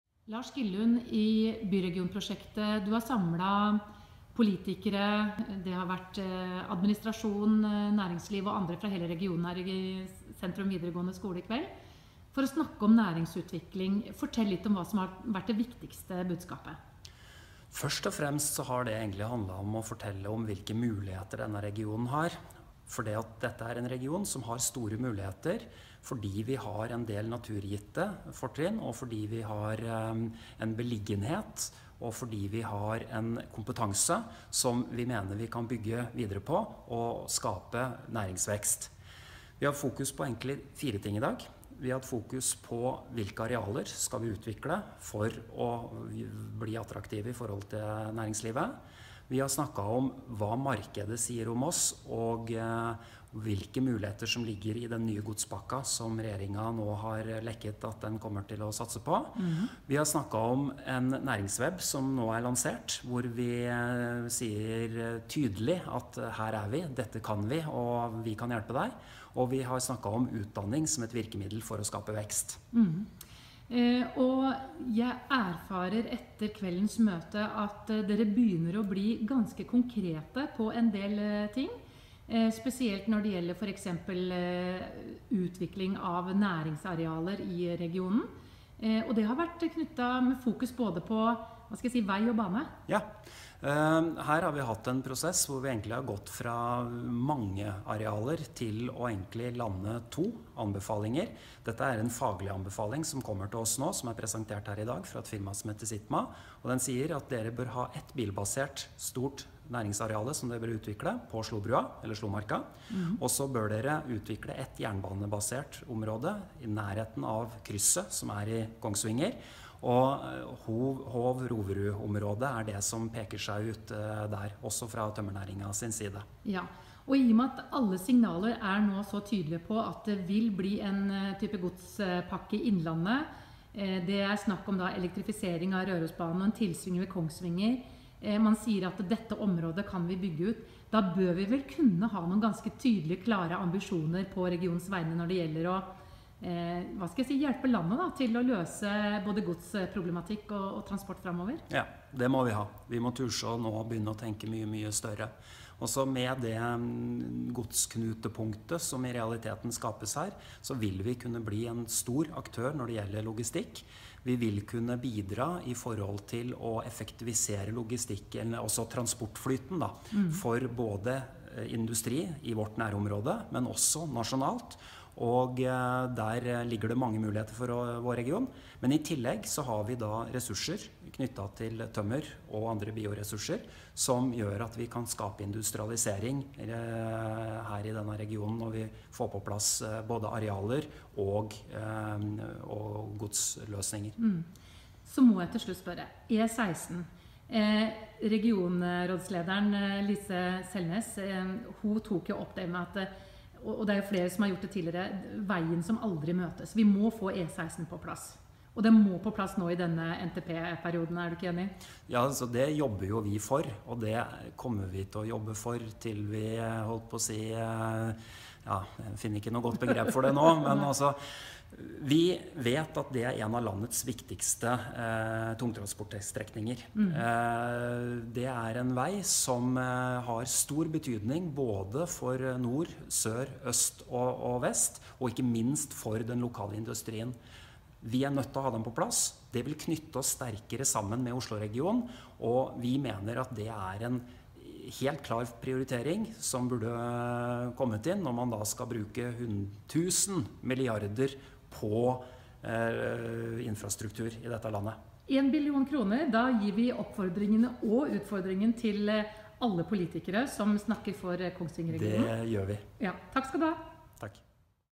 Lars Gillund i Byregionprosjektet om status og fremdrift
Byregionprosjektet arrangerte åpent møte torsdag, der det blant annet var gjennomgang av arealstrategi, ny næringsweb, godstrafikk og kompetanseutvikling.